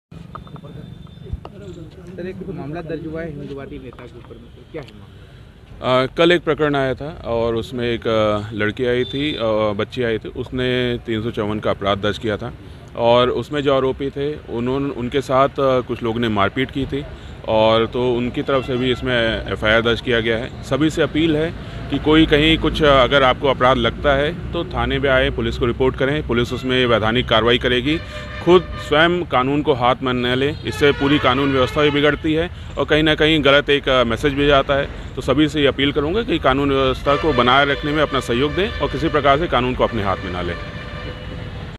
खंडवा हिंदूवादी नेता के खिलाफ केस दर्ज, बोले SP कानून अपने हाँथ में न ले, पुलिस को सूचना दें
खंडवा हिंदूवादी नेता के खिलाफ केस दर्ज होने के बाद बोले SP कानून अपने हाँथ में न ले, पुलिस को सूचना दें